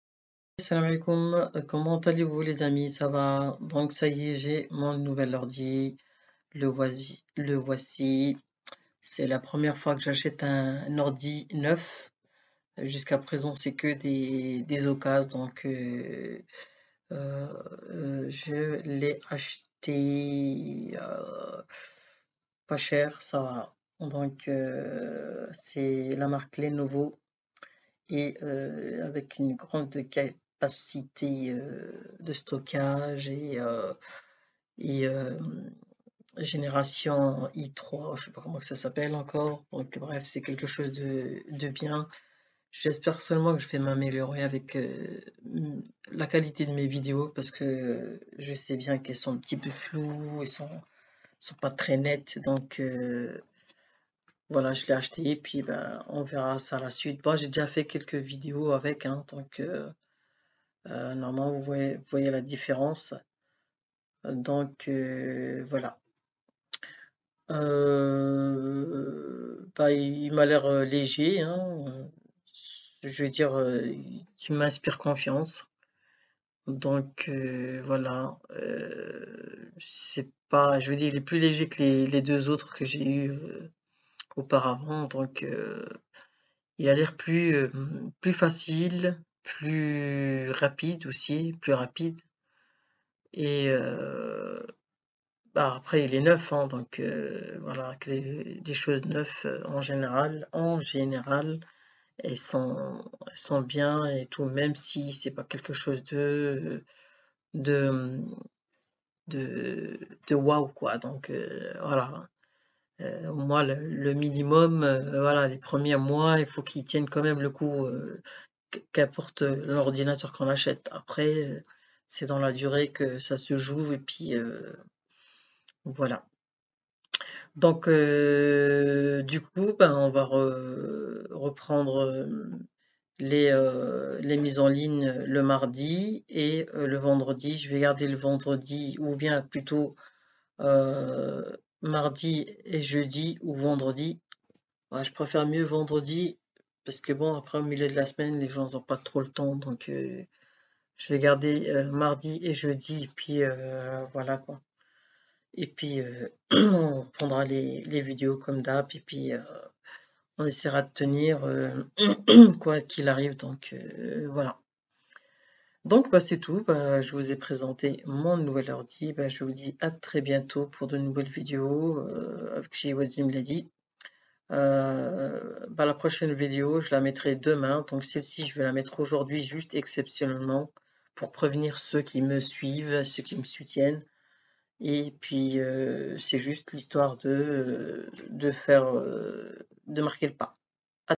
Mon Pc portable est arrivé!/ chhiwats zine bladi
Avis Favorable ! cette ordi Lenovo ! bonne qualité